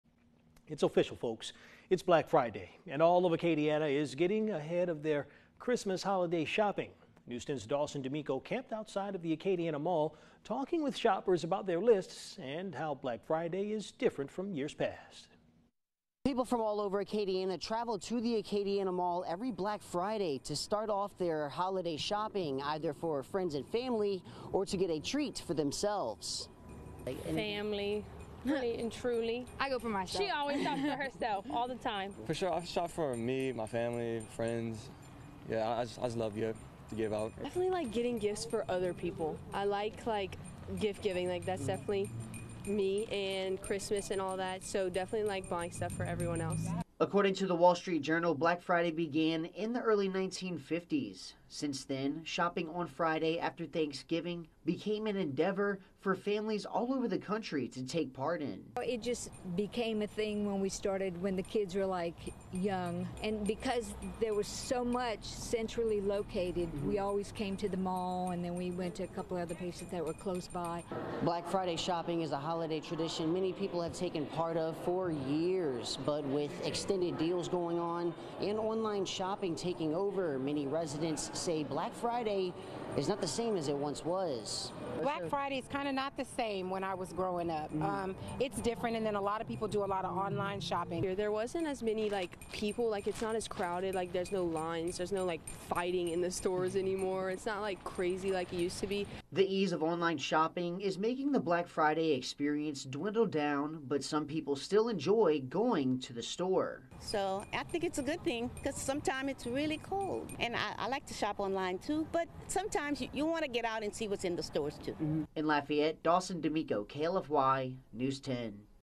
Is Black Friday shopping different than years past?
People from all over Acadiana travel to the Acadiana Mall every Black Friday to start off their holiday shopping either for friends and family or to get a treat for themselves.